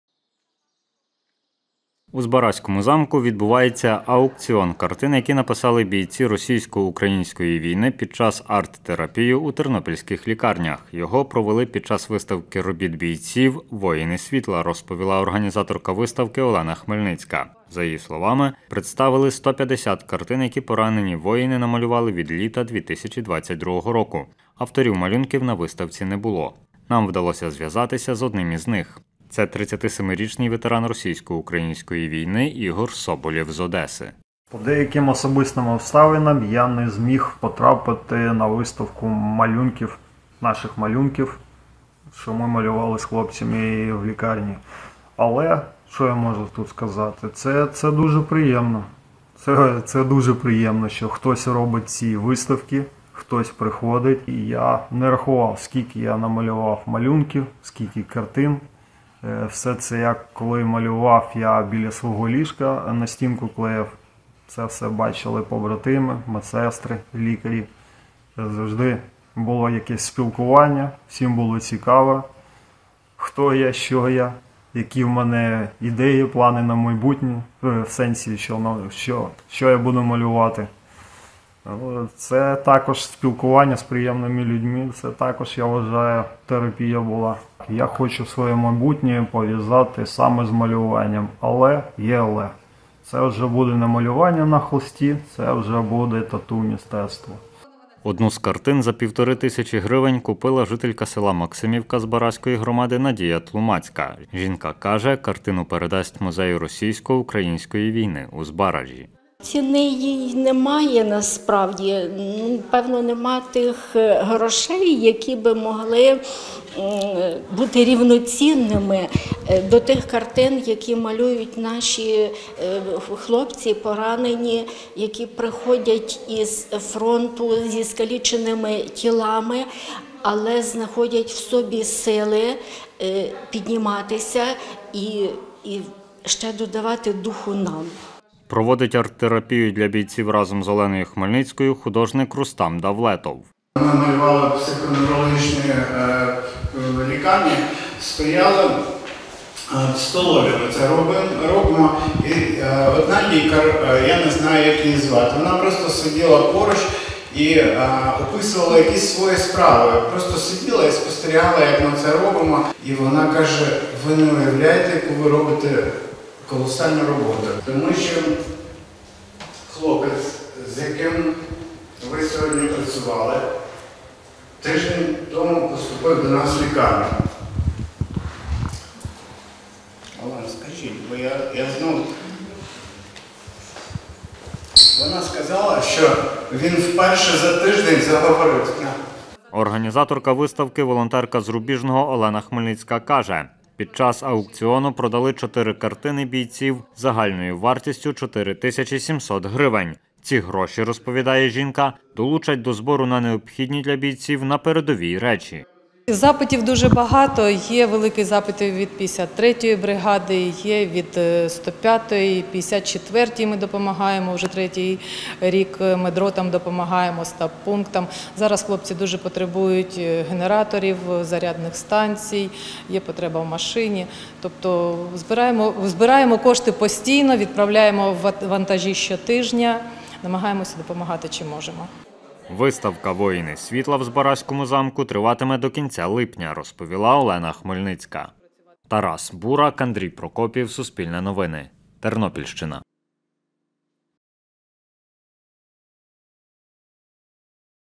Картини бійців, написані під час реабілітації, продали на аукціоні
Картини, які написали бійці російсько-української війни під час реабілітації, продали на аукціоні під час виставки "Воїни світла" в Збаразькому замку. Воїни малювали картини під час арттерапії в тернопільських лікарнях, розповіла організаторка виставки Олена Хмельницька. Скільки картин продали і куди спрямують кошти – у сюжеті.